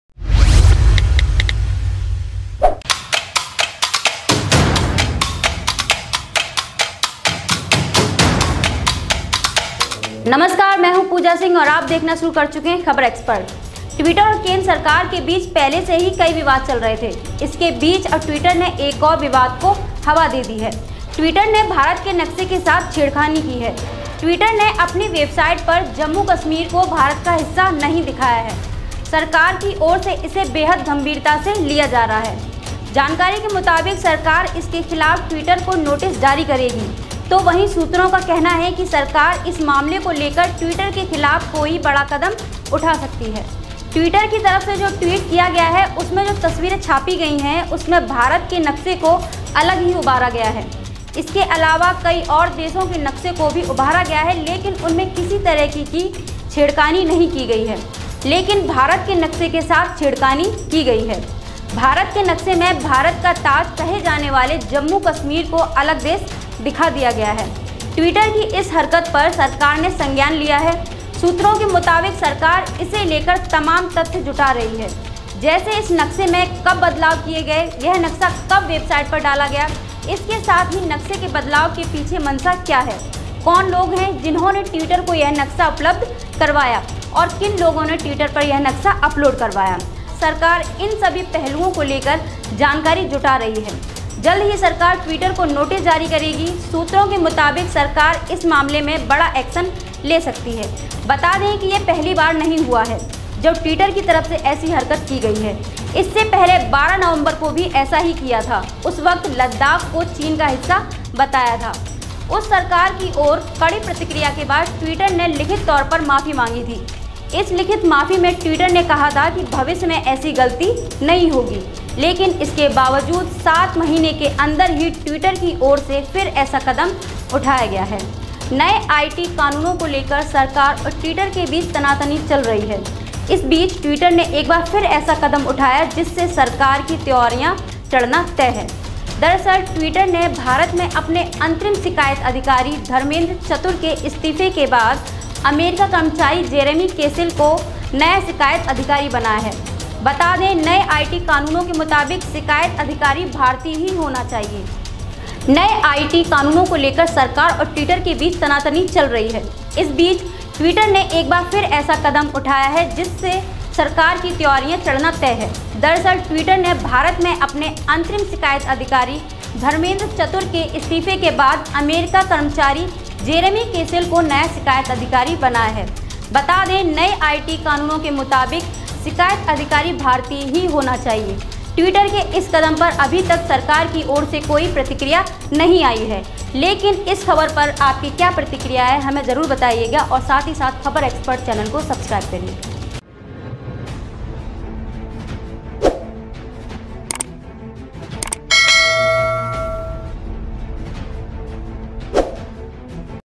Twitter ने World Map में की छेड़छाड़, INDIA के नक्शे से Ladakh को किया गायब!
ट्विटर और केंद्र सरकार के बीच पहले से कई विवाद चल रहे थे. इसके बीच अब ट्विटर ने एक और विवाद को हवा दे दी है  ट्विटर ने भारत के नक्शे के साथ छेड़खानी की है. ट्विटर ने अपनी वेबसाइट पर जम्मू कश्मीर को भारत का हिस्सा नहीं दिखाया है. सरकार की ओर से इसे बेहद गंभीरता से लिया जा रहा है. जानकारी के मुताबिक सरकार इसके खिलाफ ट्विटर को नोटिस जारी करेगी. वहीं सूत्रों का कहना है कि सरकार इस मामले को लेकर ट्विटर के खिलाफ कोई बड़ा कदम उठा सकती है.

India's map has been tampered with in the world map by Twitter. In fact, in the map of India on Twitter's website, instead of showing Jammu-Kashmir and Ladakh as part of India, a separate country has been shown.

ट्विटर के द्वारा विश्व नक्शे में भारत के नक्शे से छेड़छाड़ की गई है। दरअसल ट्विटर की वेबसाइट पर भारत के नक्शे में जम्मू-कश्मीर व लद्दाख को भारत का हिस्सा दिखाने की बजाय अलग ही देश दिखाया गया है।